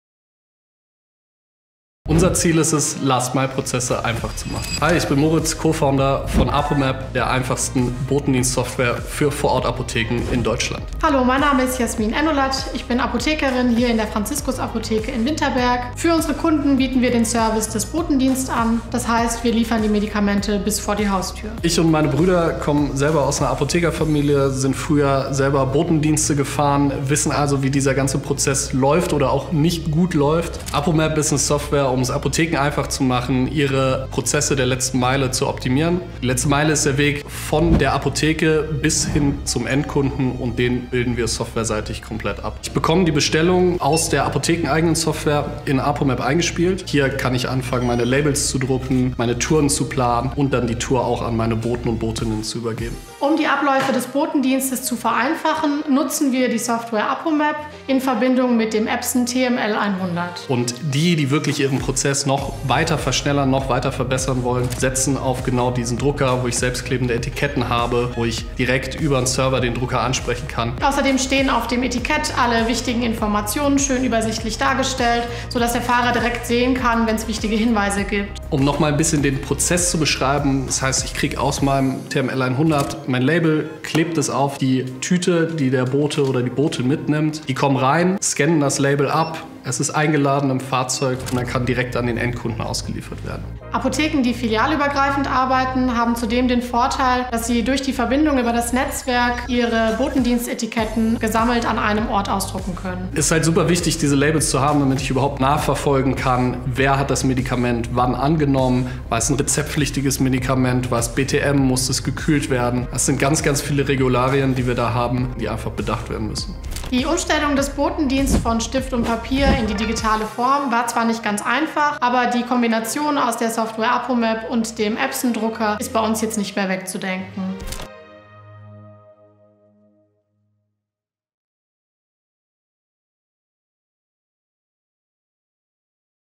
Epson im Einsatz | Etikettendruck für Lieferdienst-Software für Apotheken von apomap | TM-L100
Die apomap GmbH bietet eine Software zur Optimierung der Routenplanung von Apotheken-Lieferdiensten und wurde von Apotheker:innen für Apotheker:innen entwickelt.

Damit sämtliche Lieferungen zuverlässig abgeliefert werden können, druckt ein Epson TM-L100 Labeldrucker selbstklebende Etiketten, die jede:r der Botenfahrer:in dann auf seine anstehenden Lieferungen aufklebt. Dabei müssen gerade im Bereich Medizin und Apothekenwesen neben der reinen Lieferadresse noch eine Fülle weiterer wichtiger Informationen über das zu liefernde Medikament dargelegt werden: Wie steht es mit der Haltbarkeit, welche Vorschriften gibt es hinsichtlich der Kühlung bzw. zu der generellen Aufbewahrung, welche Prioritäten stehen bei der Auslieferung an und noch weitere mehr.